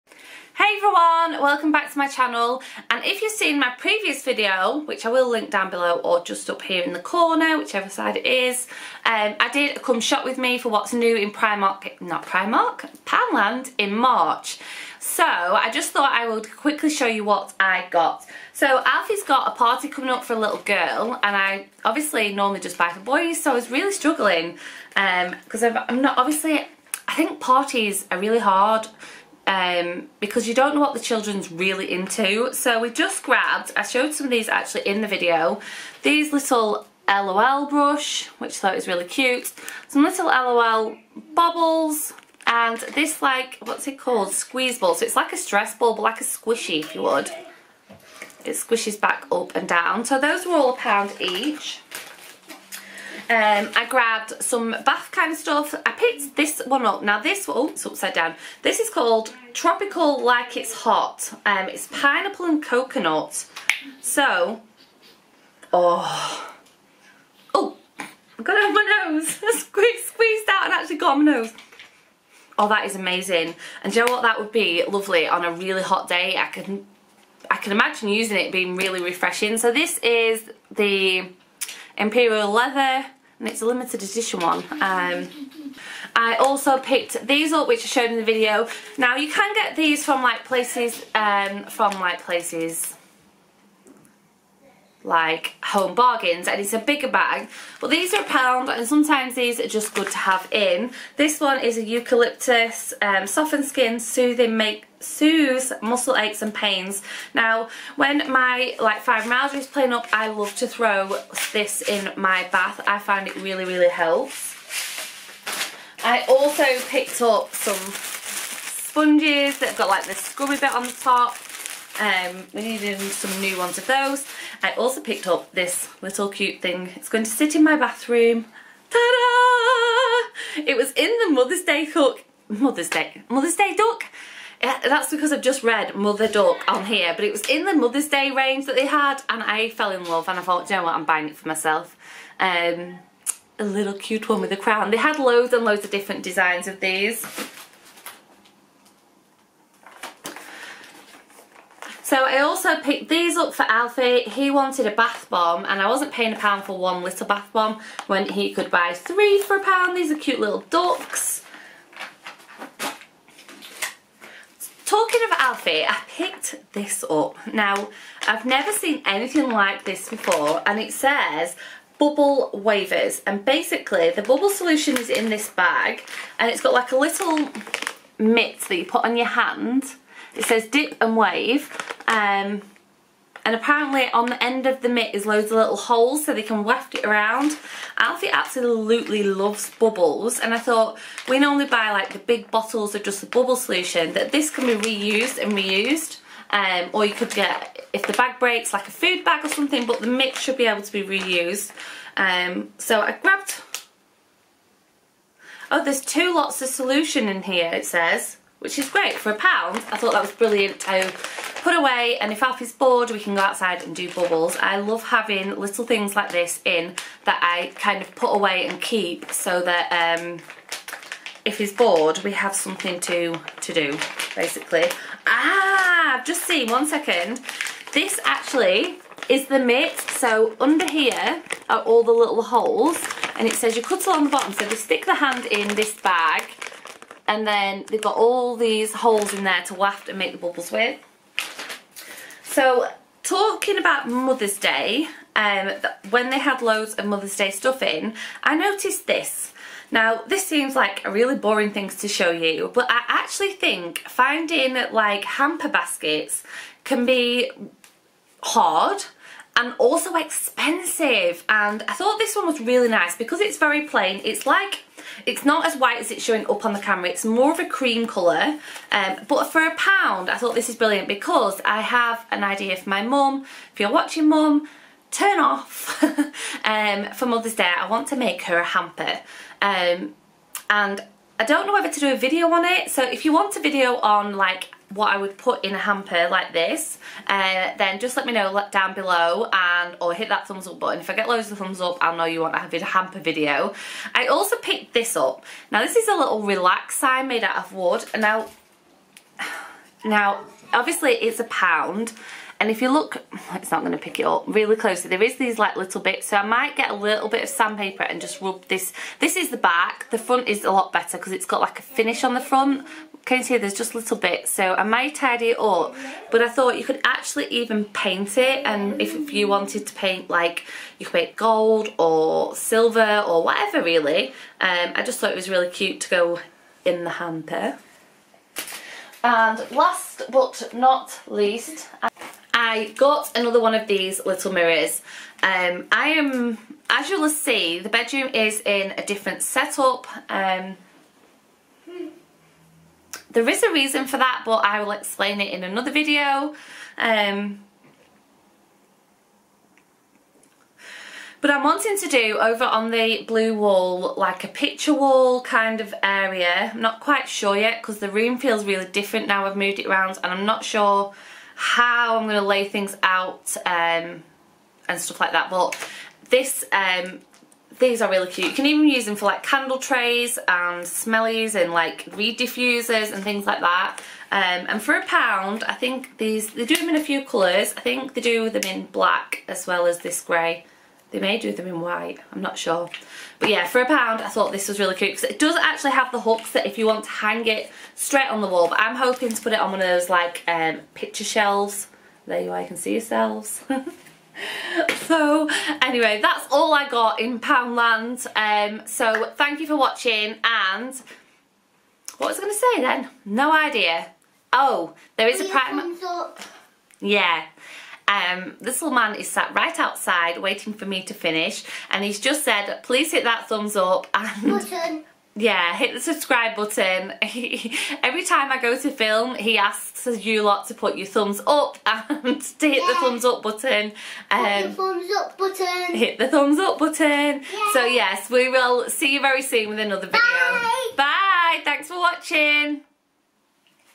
POUNDLAND HAUL FOR MARCH 2019
A small poundland haul for march 2019, hope you enjoy it .